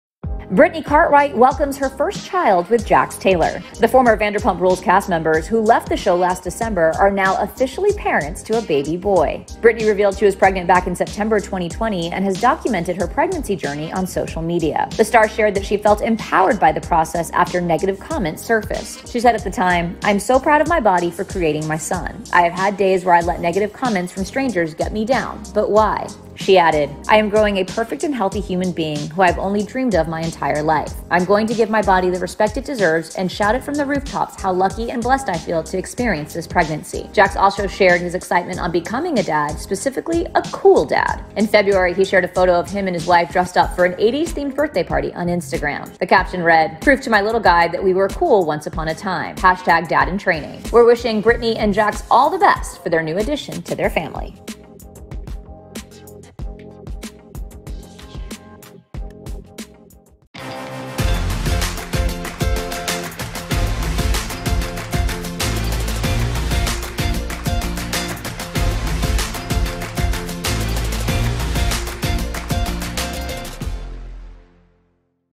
Jax Taylor & Brittany Cartwright Welcome Their First Baby | E! News
The former "Vanderpump Rules" stars announce the arrival of their baby boy, Cruz Michael Cauchi. See the first pics of Jax & Brittany's new bundle of joy!